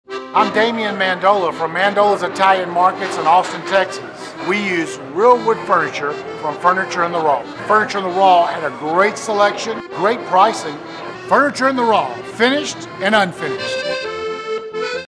Furniture in the Raw Murphy - Mandola's TV Commercial
Mandola's Italian Markets in Austin, Texas use real wood furniture in all of their dining rooms from Furniture in the Raw.

Mandola's was started almost 10 years ago by celebrity TV Chef Damian Mandola. Mandola needed durable furniture with a custom finish and chose FITR tables and chairs for the dining rooms of his three restaurants.

We're proud to say that or solid wood furniture not only looks good, but it is "restaurant tough."